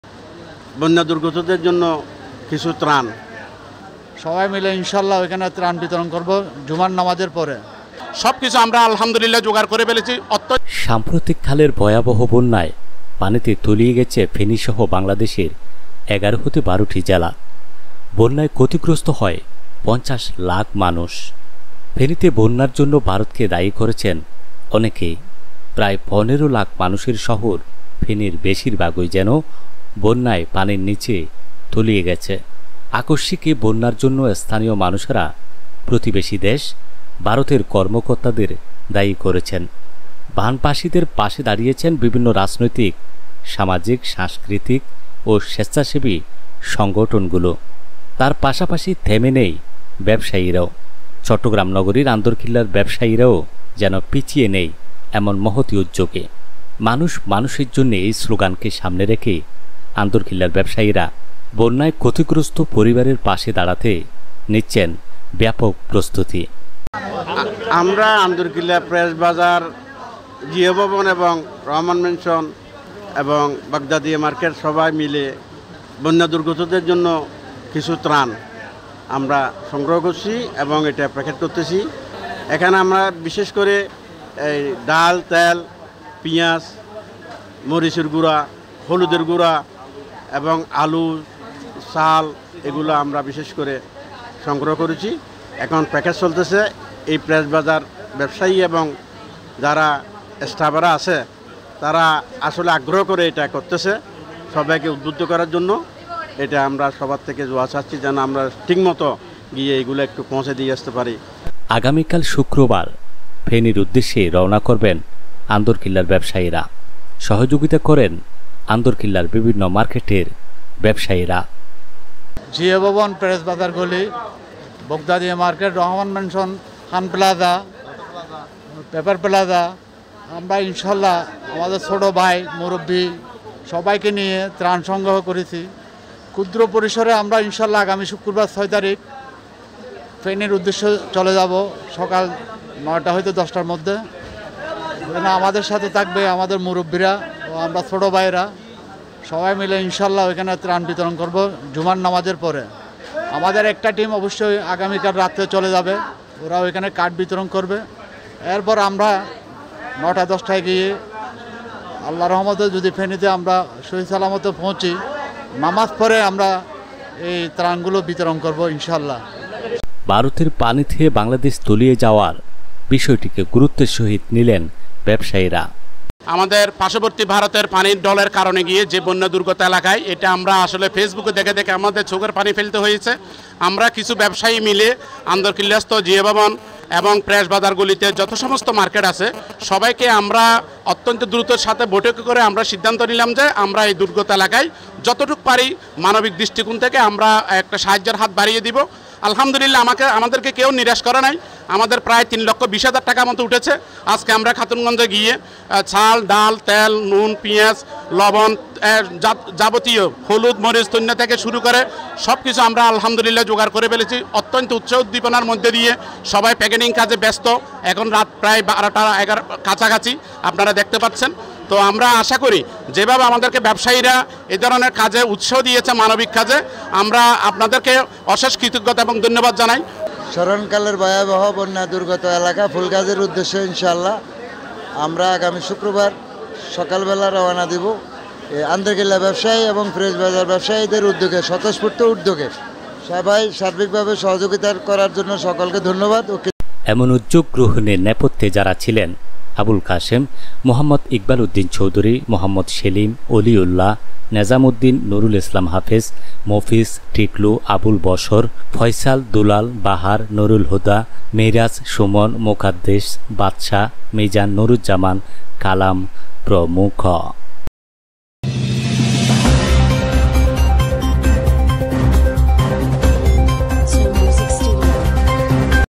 ফেনীতে ত্রাণ সহায়তা প্রদান করবেন চট্টগ্রাম নগরীর আন্দরকিল্লার ব্যবসায়ীরা
Chatgar Media | C Media | C News | News Today | News Update
পেইজবুক পেইজ